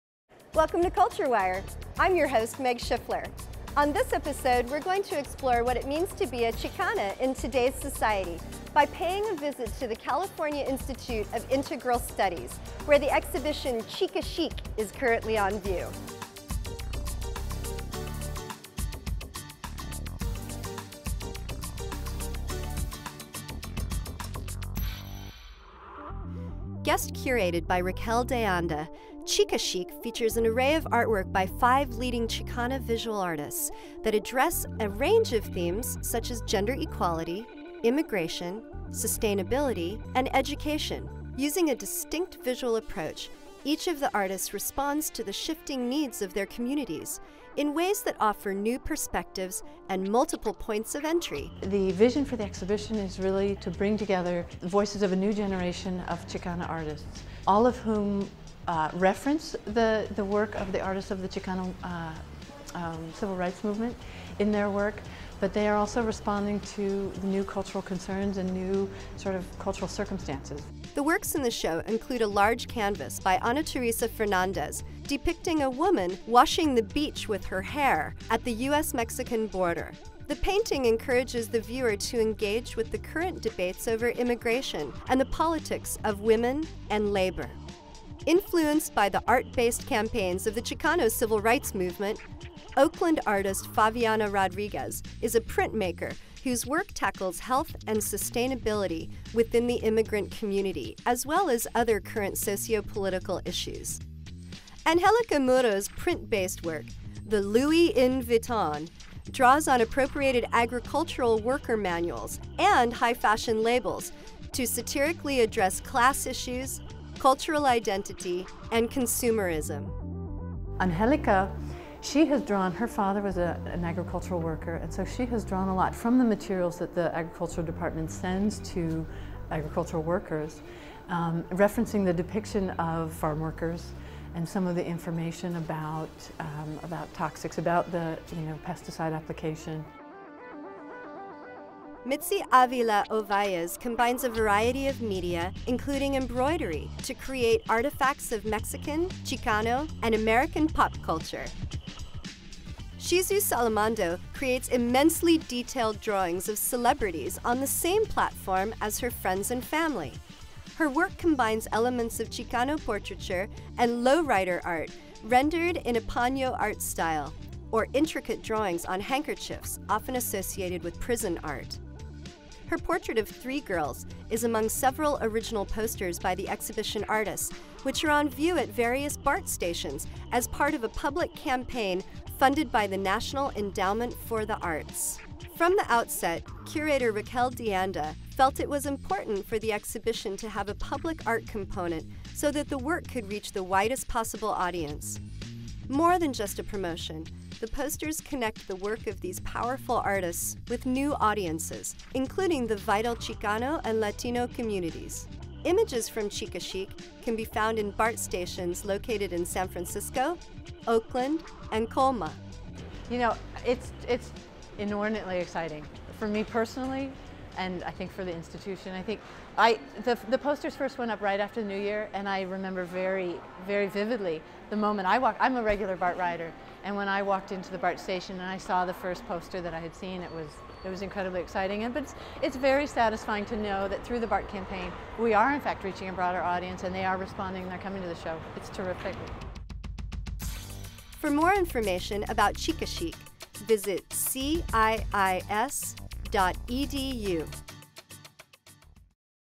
Culture Wire: Chica Chic
Chica Chic exhibits work from five Latina artists living in the Bay Area. Stop by the California Institute of Integral Studies at the corner of 8th and Minna Sts.